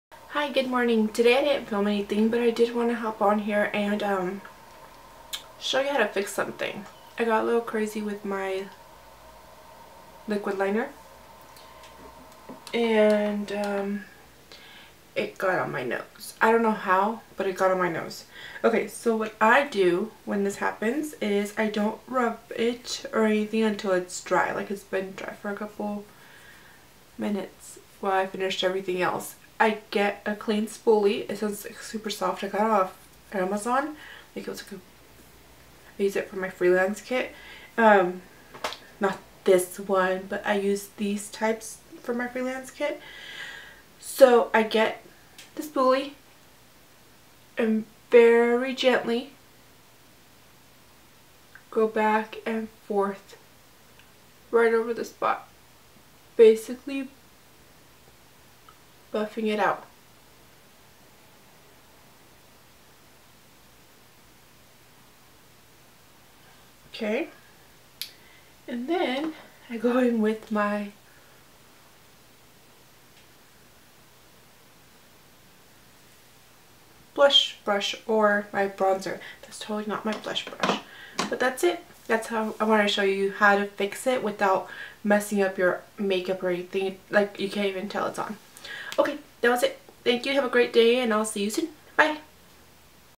Super Quick Fix
So I'm very careless while applying my makeup and this actually happens a lot! This works for eyeliner or mascara just be very careful to be GENTLE! But in a pinch it is super helpful. Do you enjoy long videos or just short tip videos like this? I need some sense of direction here😣😅.